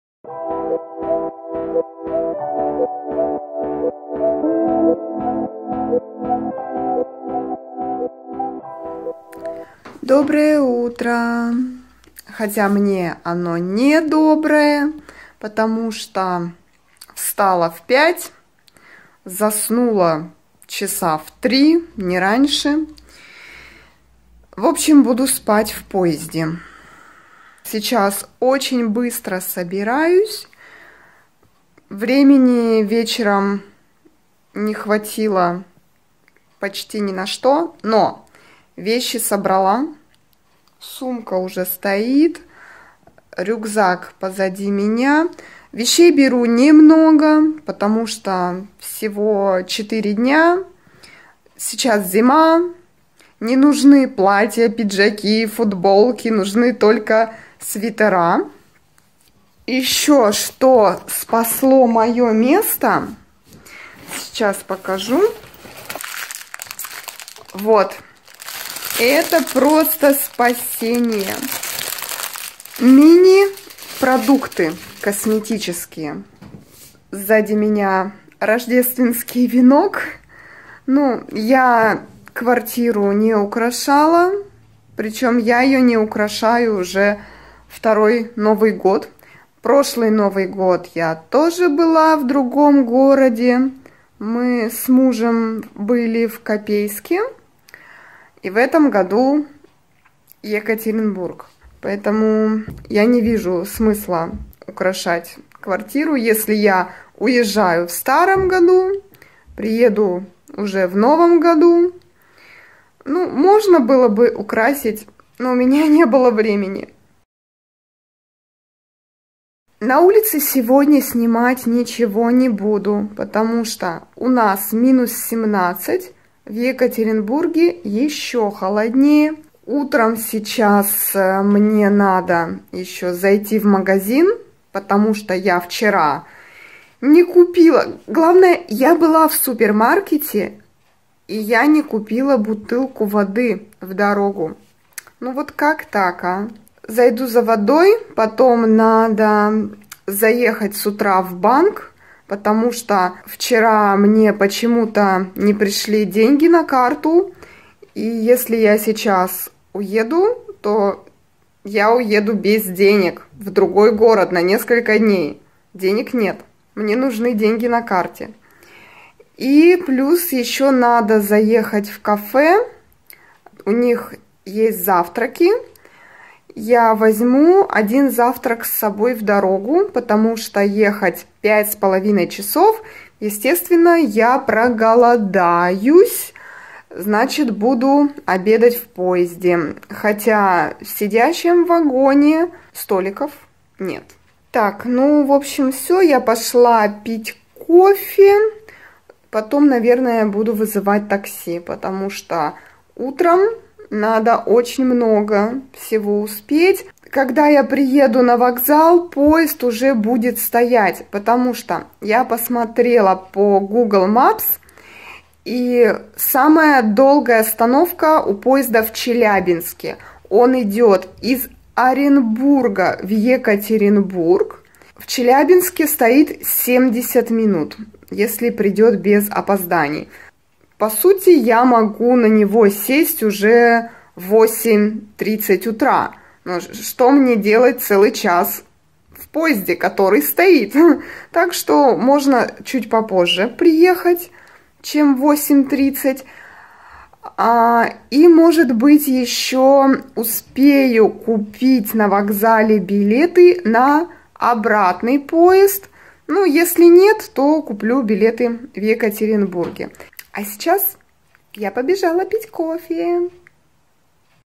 30.12.2019. The morning before the train. Утро перед поездом.
Interesting moments about the Russian language that you can hear in this video.
1) The word order is different than in the English language, therefore, almost all my phrases are translated literally.
2) Double negative. For example, времени не хватило почти ни на что - the time WASN'T enough almost for NOTHING.
3) A phrase "I don't need" sounds like "isn't needed / aren't needed" - не нужно / не нужен / не нужна / не нужны in Russian.
4) "Last year" sounds like "in the last year" - В прошлом году in Russian.
5) To leave by transport - уезжать, to come / to arrive by transport - приезжать.
6) "To stop by the bank / the cafe" sounds like "to stop by IN / INTO the bank / the cafe" - заехать В банк / В кафе.
7) In the Russian language, money can COME to your card. If you didn't have money on your card before and now you have it (someone sent it to you), you can say "money came to the card" - деньги пришли на карту.
8) If you're about to go to do something, you can describe it like you have left already - "I WENT to drink coffee" - я ПОШЁЛ/ПОШЛА пить кофе.
9) In the Russian language, the train can STAND - стоять at the station.
10) In the Russian language, when the train GOES, it ИДЁТ.
11) In the Russian language, if I BOARD the train, then I literally "SIT ON the train" - я САЖУСЬ НА поезд.
12) If you're about to go to do something REALLY FAST, you can say that you RAN to do it - "I RAN to drink coffee" - я ПОБЕЖАЛ/ПОБЕЖАЛА пить кофе.